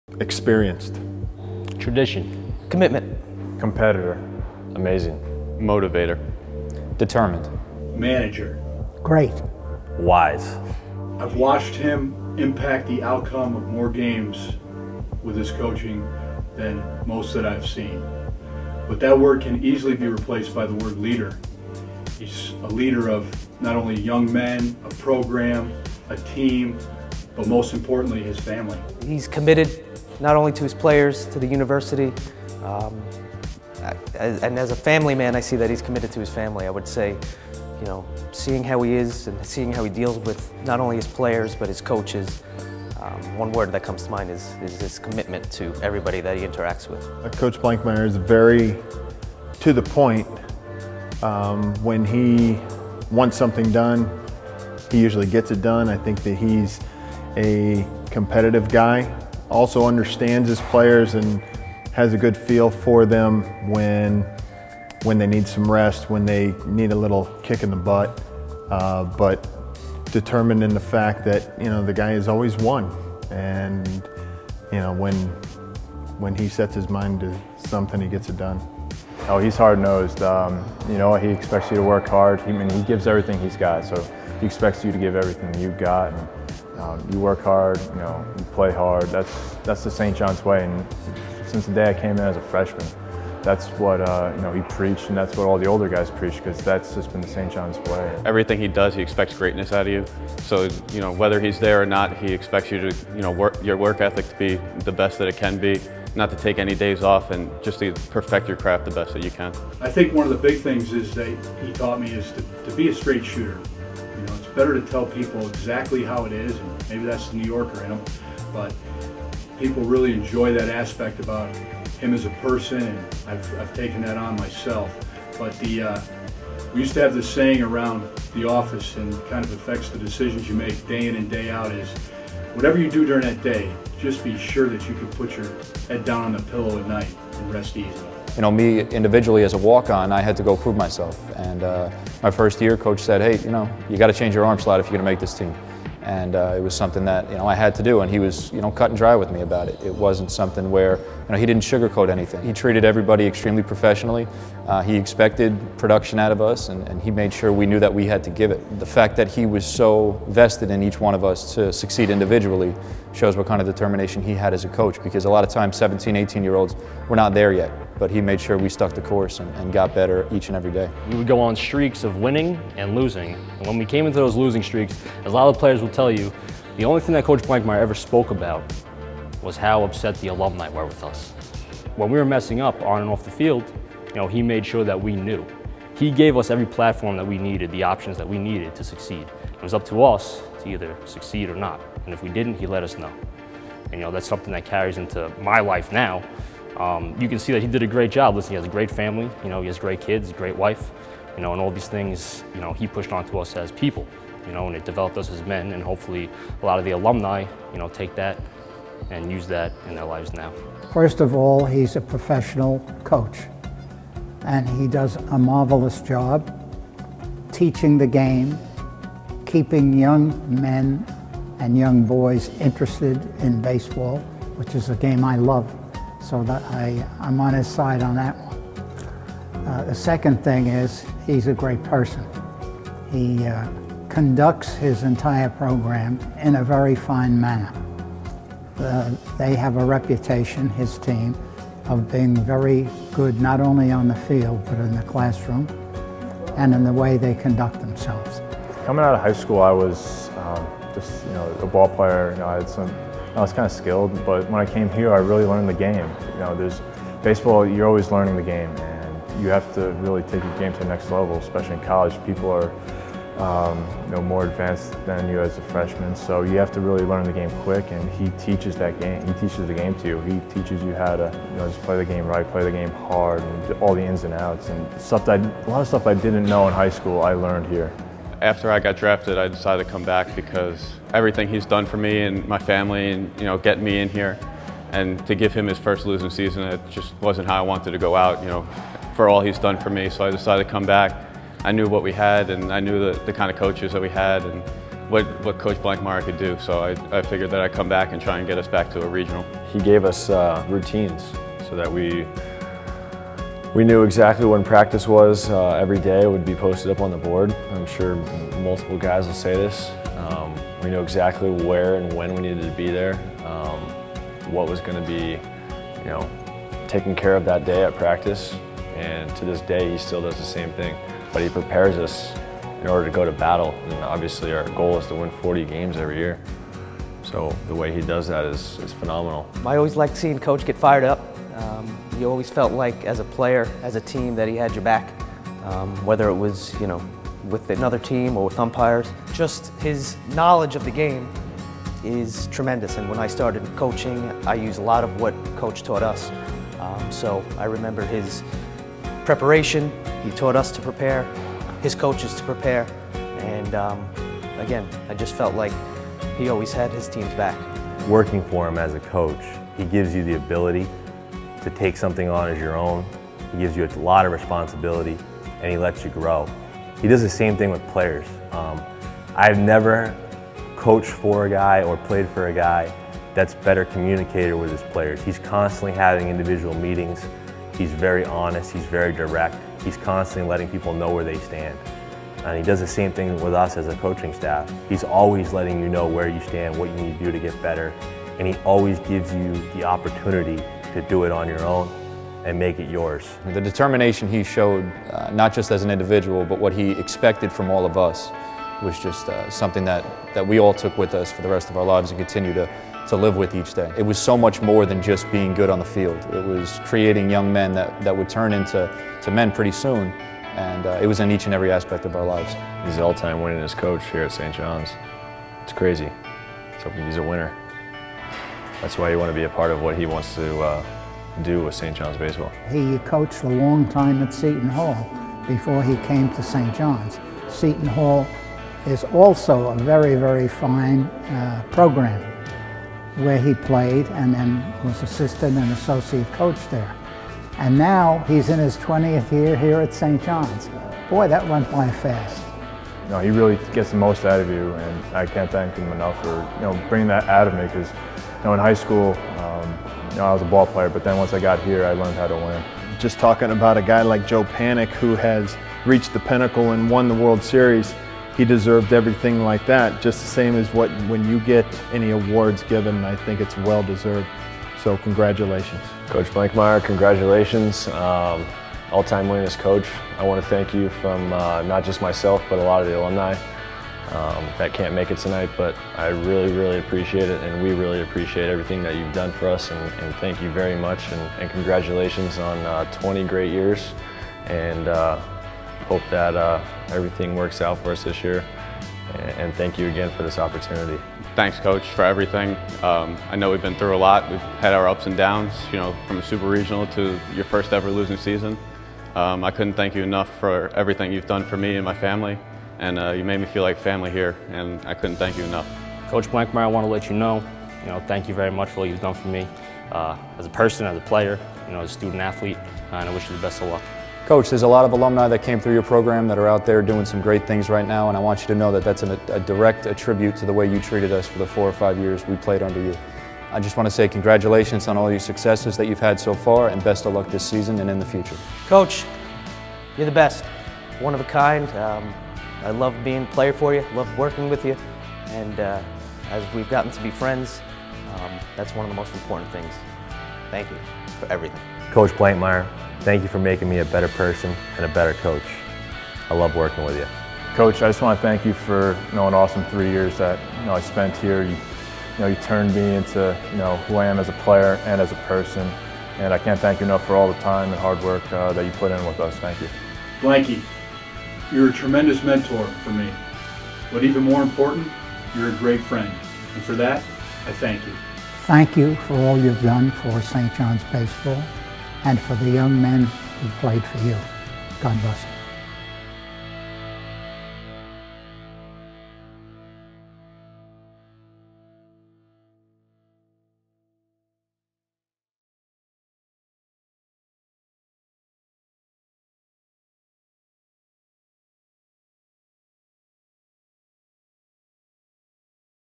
St. John's Baseball Coach Ed Blankmeyer Tribute
As part of Coach Ed Blankmeyer's special tribute on Jan. 23 during the annual baseball banquet, a video was presented to him featuring his former players, coaches, colleagues and friends.